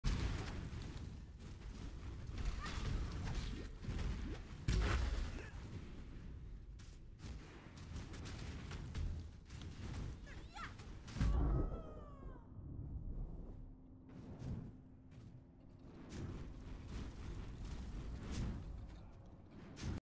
Remember lessons from the past or reproduce it until you learn
Still ignoring the Atlas's map specifications (Reflected Elements Damages) but playing more defensive... Same result at the end.
No warning, no visuals effects, instant pure damages leads to a fast & clean suicide...